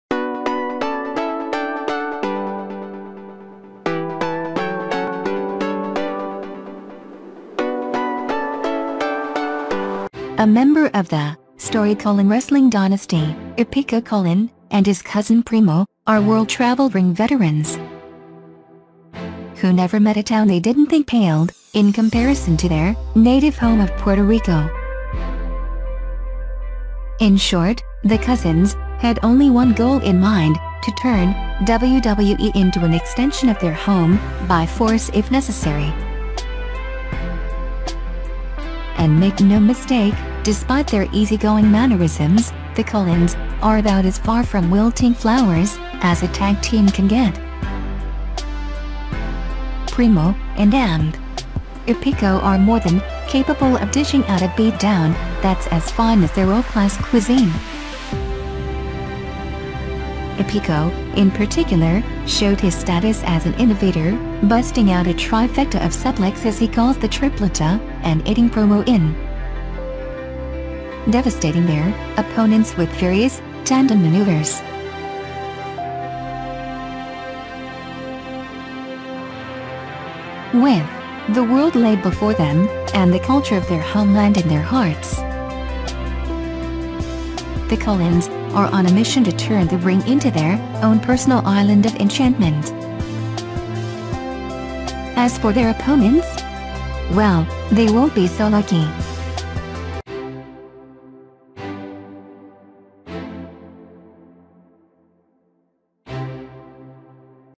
EPICO COLON BIOGRAPHY
A member of the storied Colon wrestling dynasty, Epico Colon and his cousin Primo, are world-traveled ring veterans who never met a town they didn’t think paled in comparison to their native home of Puerto Rico. In short, the cousins had only one goal in mind: To turn WWE into an extension of their home, by force if necessary.

And make no mistake, despite their easygoing mannerisms, The Colons are about as far from wilting flowers as a tag team can get. Primo & Epico are more than capable of dishing out a beat down that’s as fine as their world-class cuisine. Epico, in particular, showed his status as an innovator, busting out a trifecta of suplexes he calls the “Tripleta” and aiding Primo in devastating their opponents with various tandem maneuvers.

With the world laid before them and the culture of their homeland in their hearts, The Colons are on a mission to turn the ring into their own personal, Island of Enchantment. As for their opponents? Well, they won’t be so lucky.